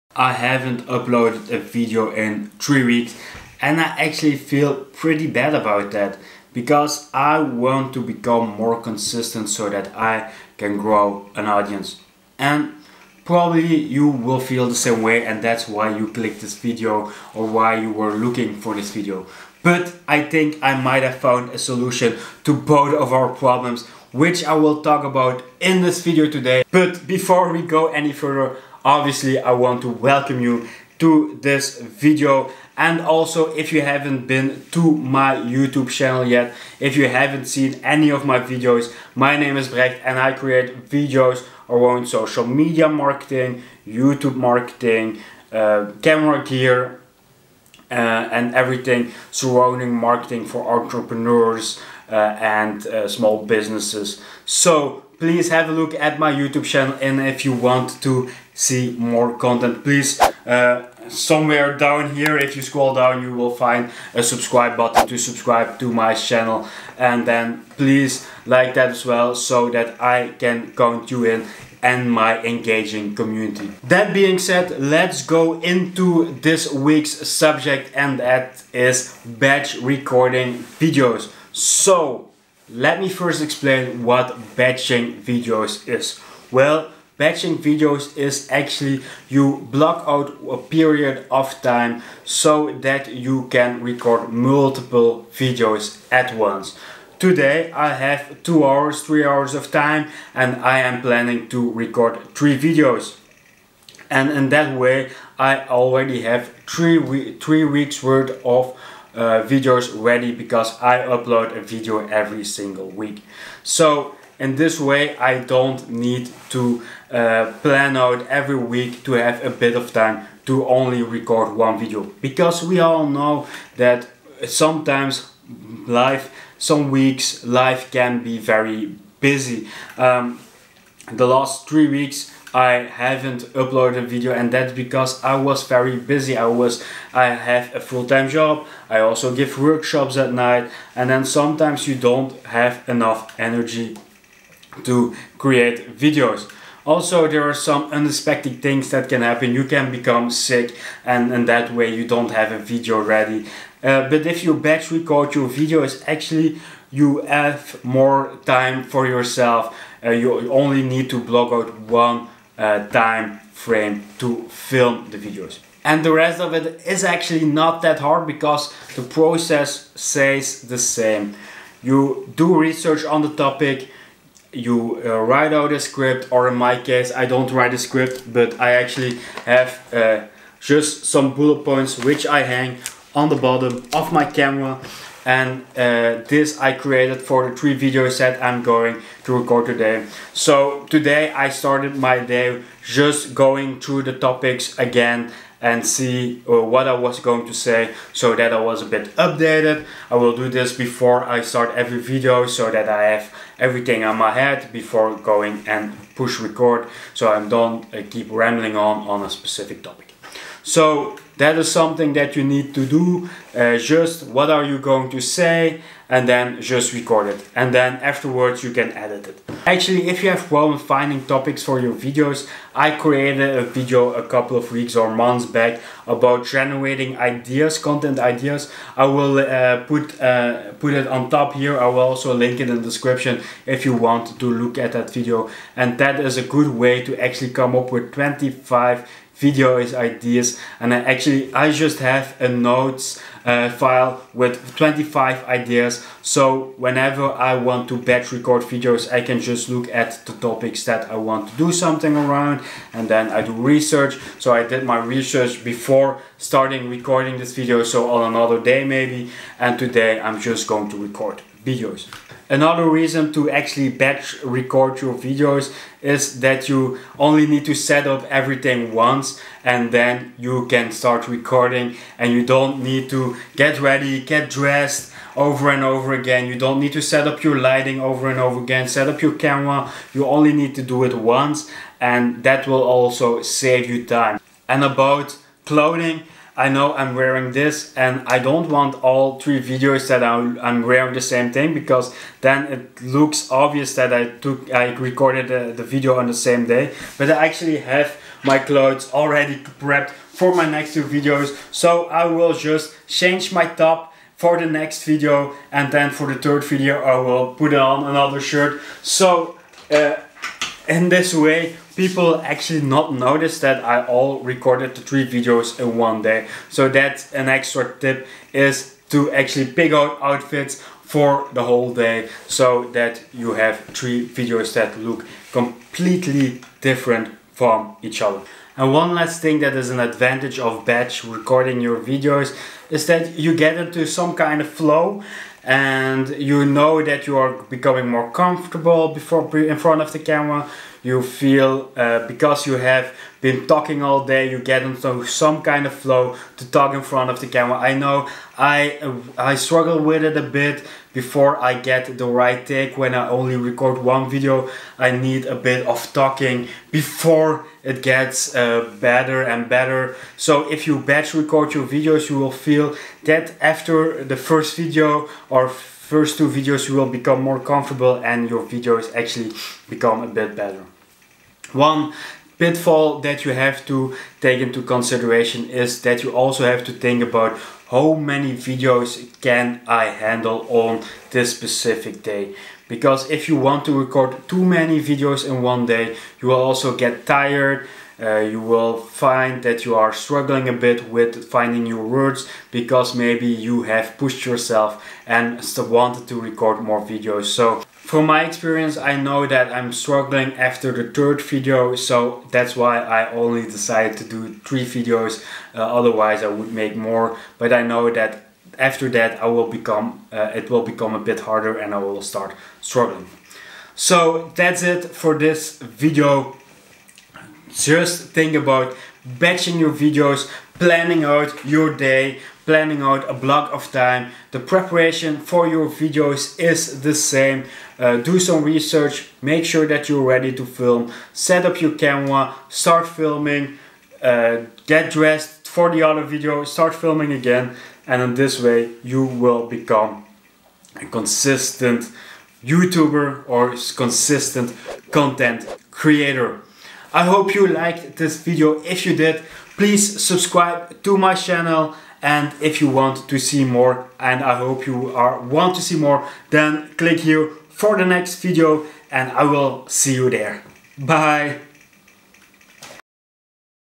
Batch Record Videos - GET CONSISTENT ON YOUTUBE
Having trouble getting consistent on YouTube? I feel you! I might have a solution to our problem. Batch record your videos. Yes, block out a time and record more videos which you upload over time. Pretty cool huh?